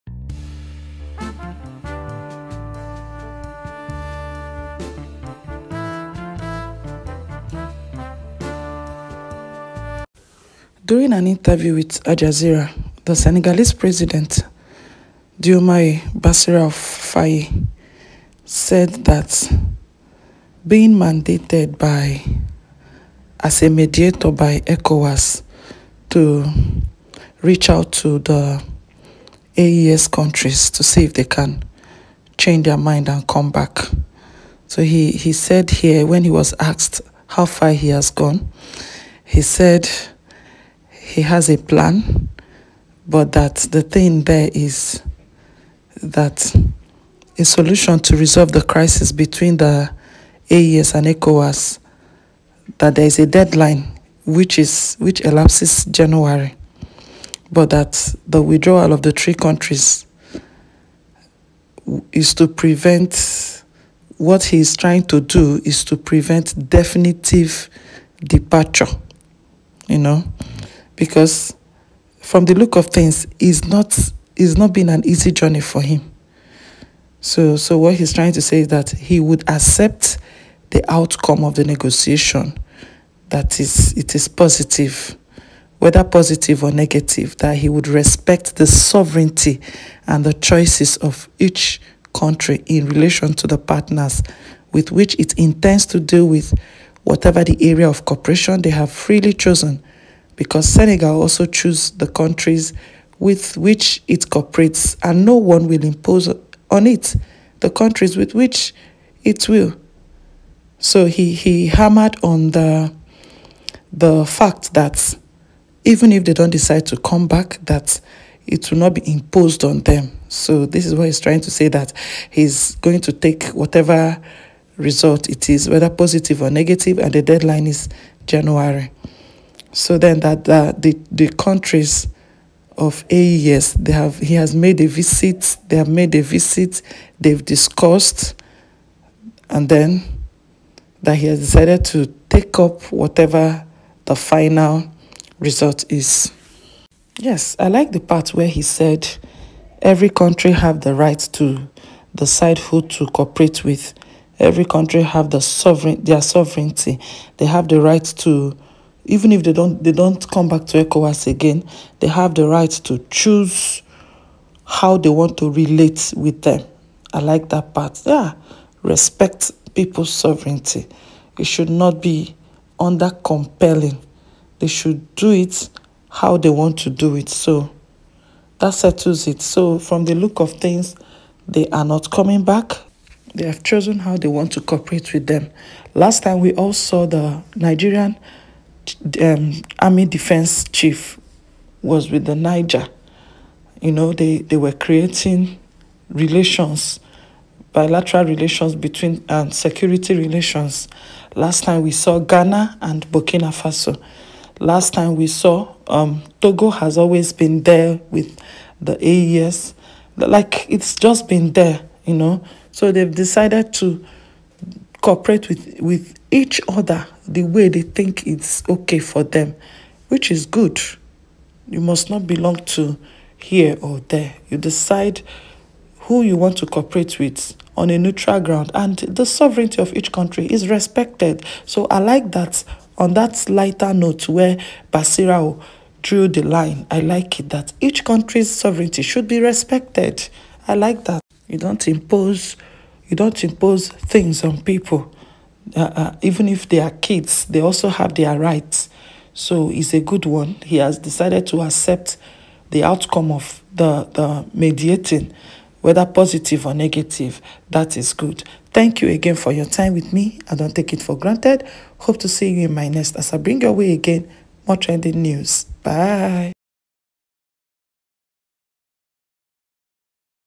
ECOWAS-AES: "Burkina faso, Niger, Mali Are Still Ecowas Members" President Diomaye Faye Bassirou
Speaking in an interview the Senegalese President Diomaye Faye Bassirou says he will accept the outcome of the mediation misson for Ecowas and the three countries even its positive or negative.